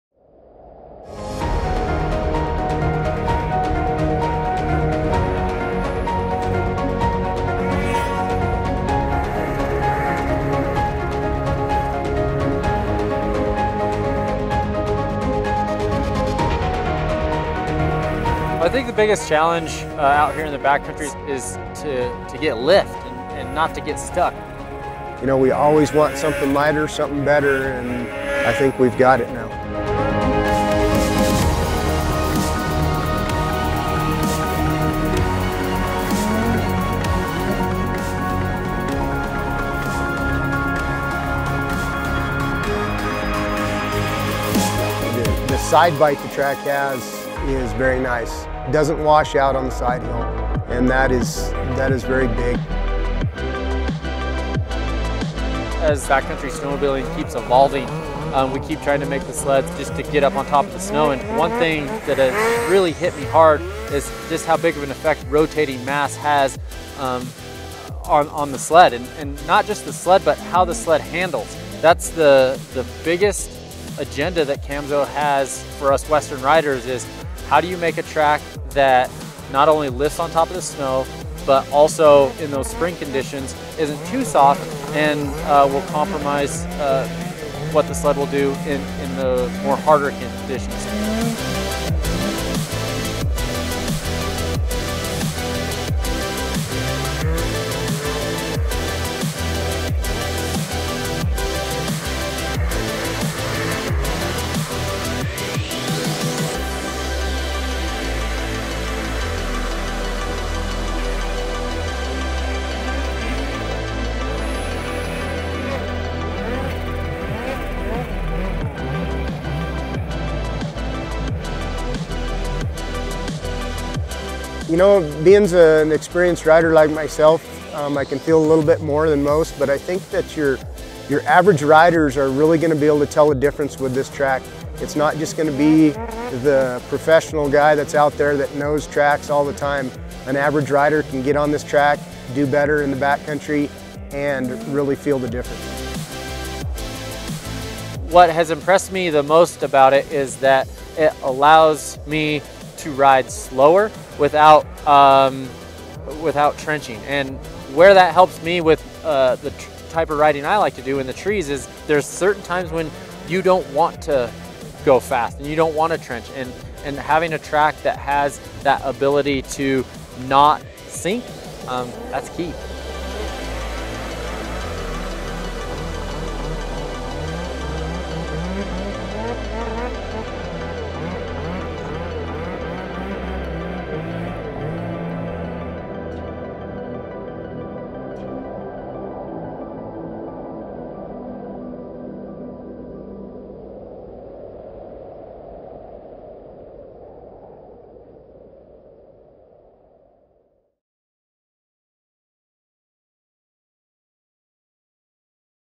Camso Conquer 280 | Chris Burandt track review
The Camso Conquer 280 is the lightest mountain snowmobile track in the market. It offers better traction, flotation, and agility in deep snow. Learn why Dennis Durmas, Camso dealer, and @ChrisBurandt  pro snowmobiler, enjoy it.